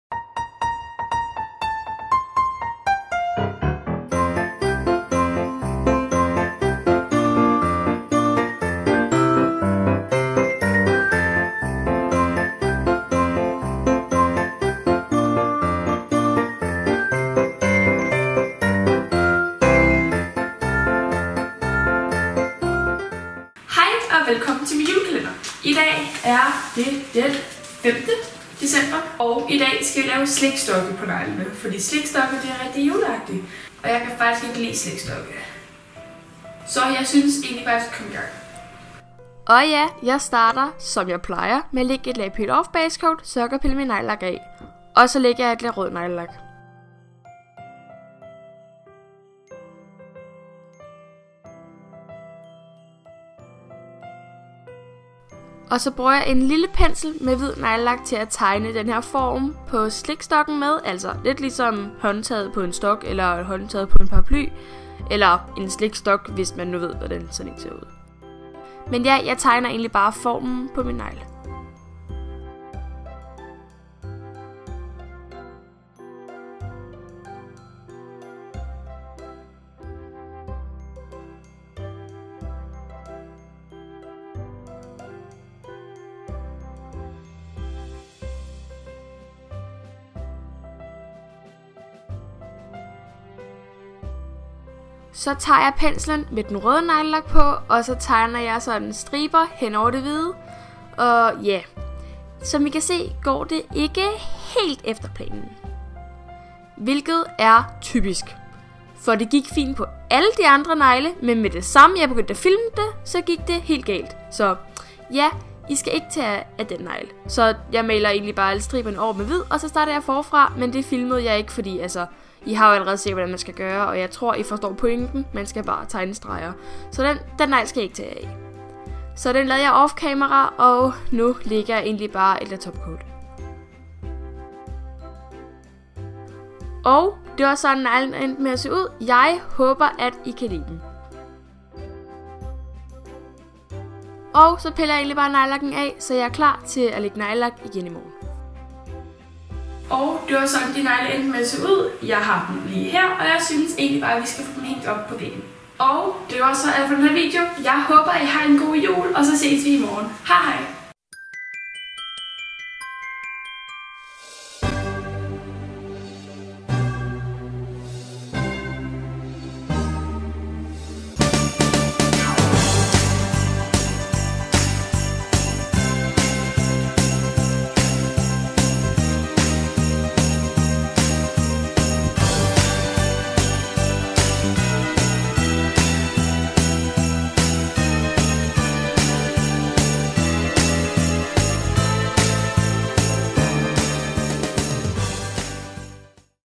5. december /// Slikstokke
Tak for at du så med.

Neglelakker:

Basecoat= Unt - Ready for takeoff
Topcoat= Seche vite - Dry fast topcoat
Hvid= Opi - Alpine snow
Rød= Christian Louboutin - Rouge Louboutin

❤️❤️INFO❤️❤️

---alder= 14---

---kamera= Panasonic hc-vxf999---

---Facebook= katrines negle & katrines snegle---